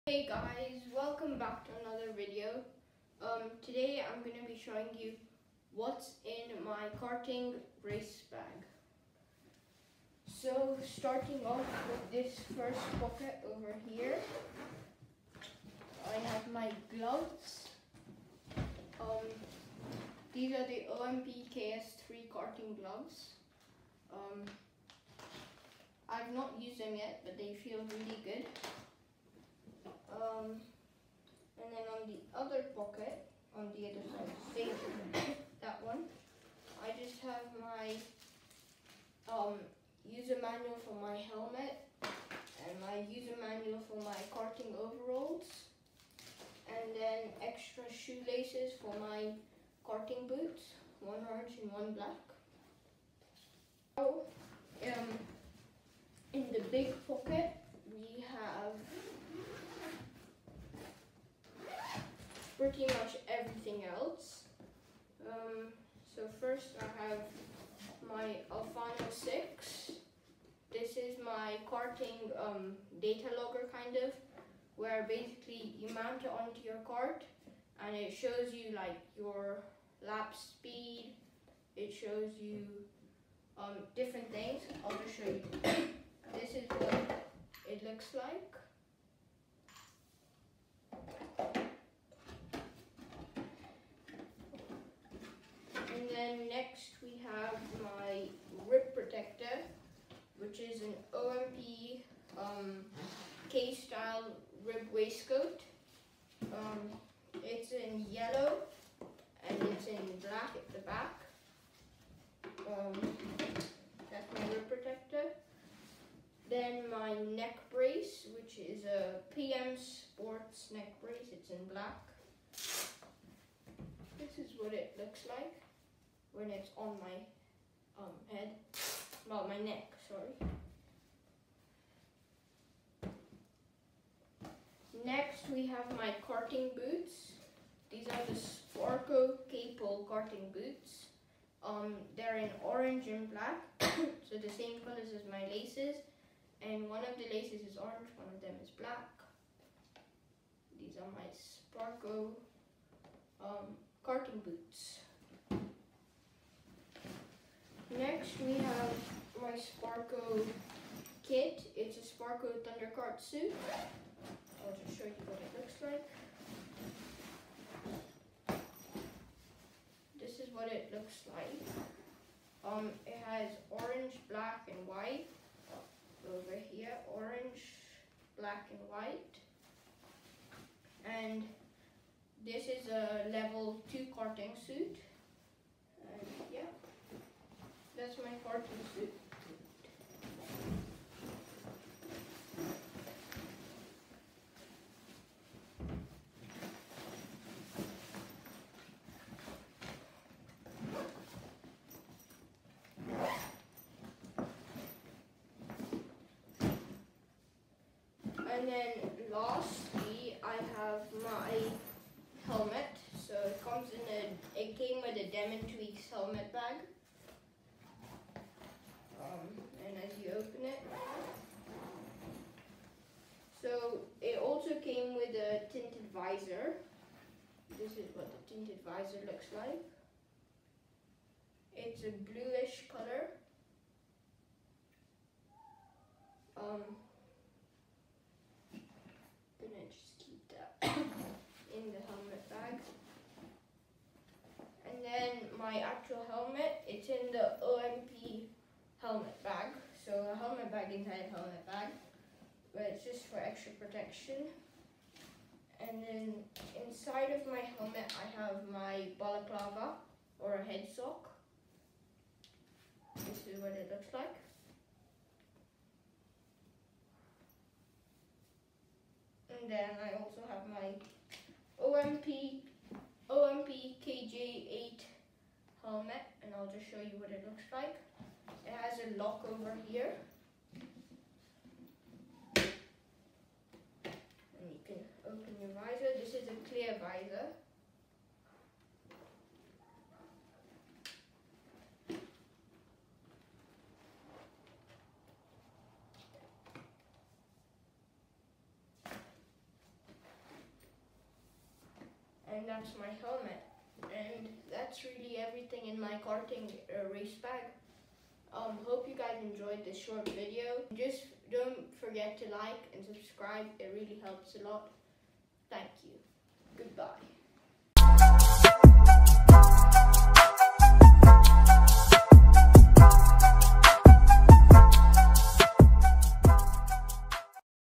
What’s in My Karting Bag?
In this video I will be showing you what is inside my Karting Bag for this year as I am just starting Karting.